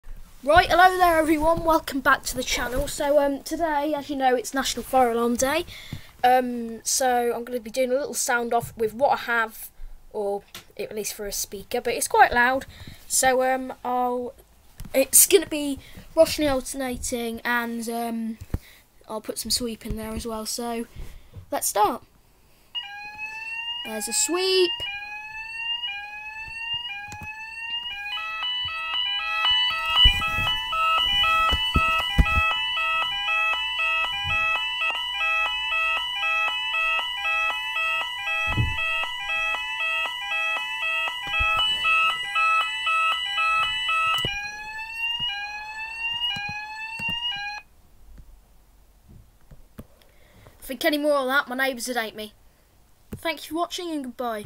Happy National Fire Alarm day!
just a little sound-off to celebrate national fire alarm day